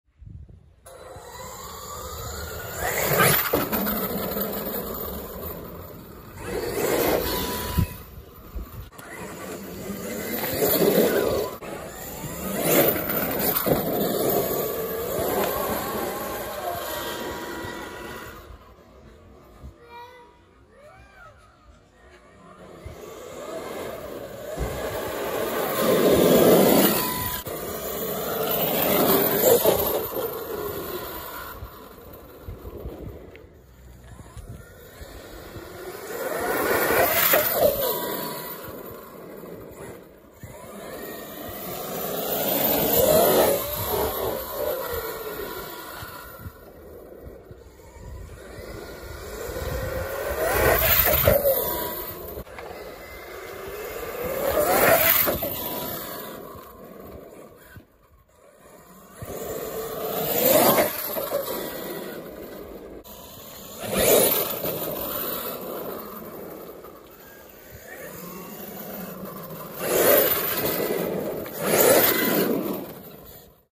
RC-Vloggen #3 - Arrma Outcast 8s
Kort klipp på nya bilen påväg ner till parkeringen 😃